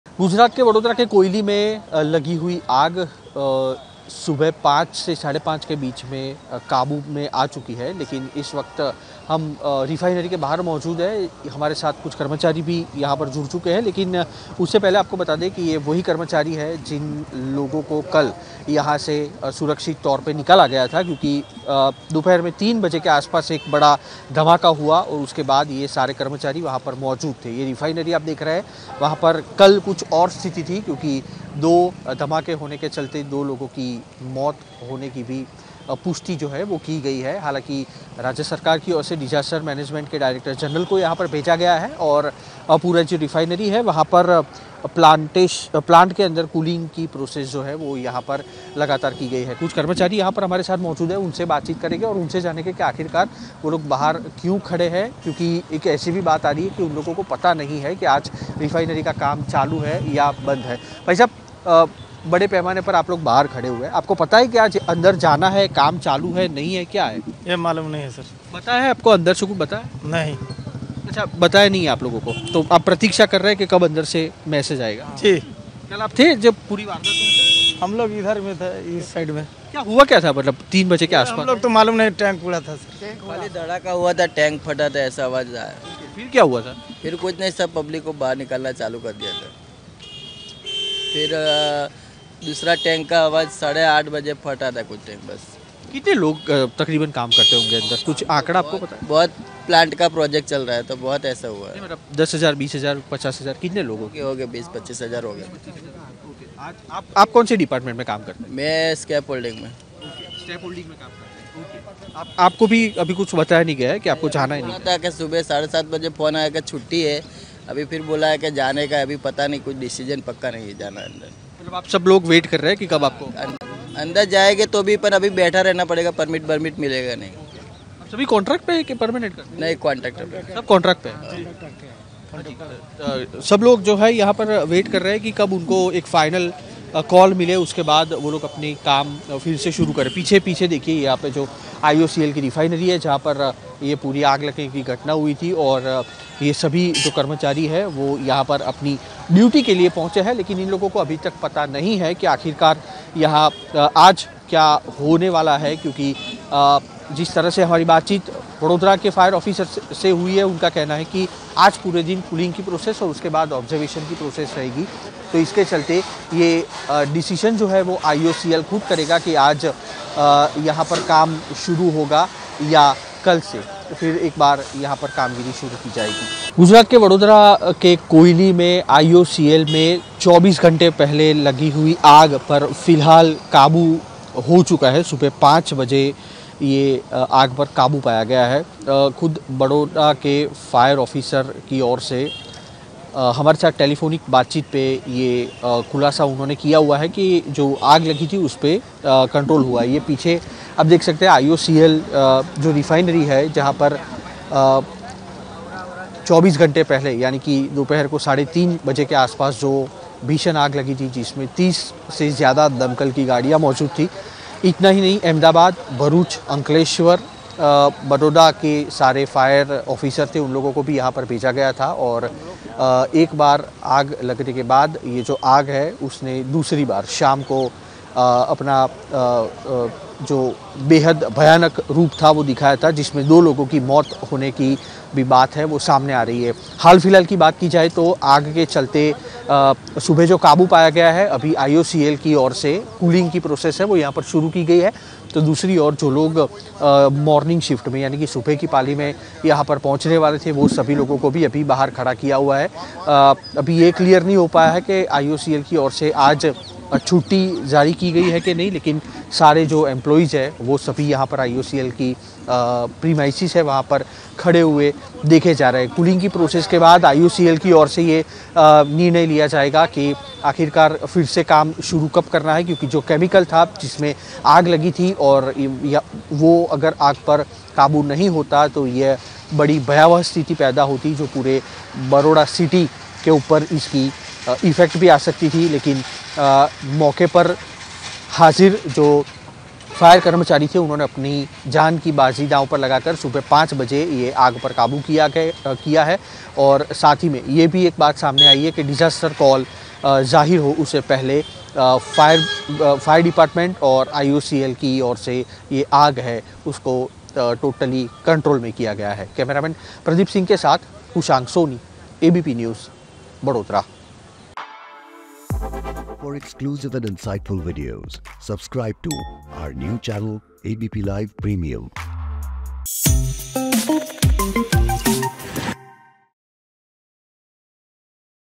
Vadodara Fire: ववडोदरा की रिफाइनरी में धमाका, 15 घंटे बाद आग पर काबू...2 की मौत | ABP LIVE
डीसीपी ट्रैफिक पुलिस ज्योति पटेल ने कहा, "रिफाइनरी में बचाव अभियान जारी है। हमें इस घटना में किसी के गंभीर रूप से घायल होने या मौत की कोई सूचना नहीं है... मार्ग पर यातायात की आवाजाही बहुत कम है, इसलिए हमें मार्ग बदलने की जरूरत नहीं पड़ी।" स्थानीय बीजेपी विधायक धर्मेंद्र सिंह वाघेला ने कहा, "गांव के सरपंच ने मुझे फोन करके घटना की जानकारी दी... मैंने संबंधित अधिकारियों से आग लगने के कारणों के बारे में जानकारी लेने की कोशिश की... मैं स्थिति का आकलन करने के लिए अंदर जाऊंगा और अधिकारियों से बात करूंगा। हमने कलेक्टर, डिप्टी कलेक्टर, पुलिस और आईओसी अधिकारियों के साथ एक तत्काल बैठक भी बुलाई है ताकि यह सुनिश्चित किया जा सके कि ऐसी घटना फिर न हो।"